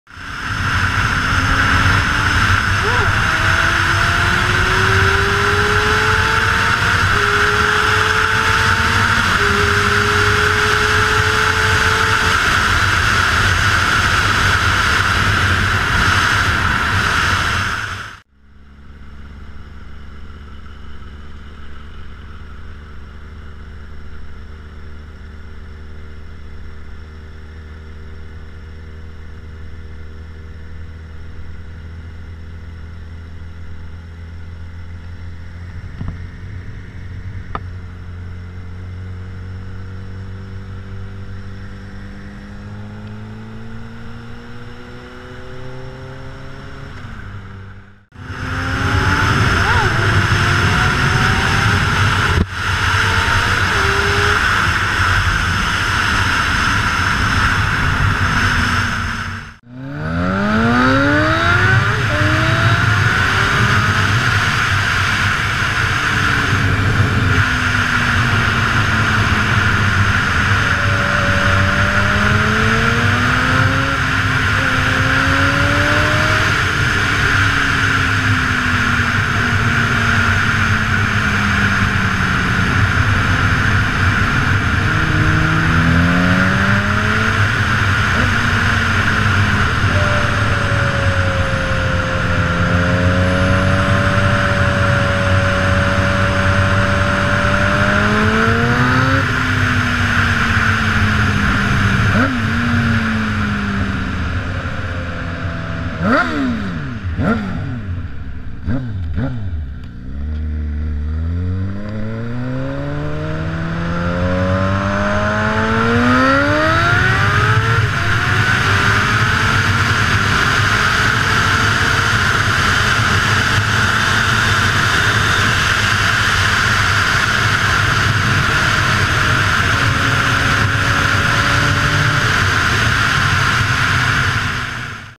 zx6r go pro hero 3 messing around on the weekend
just a short clip of some of the riding we did last weekend. nothing too special there was a nasty wreck with a battery truck.hope the people were ok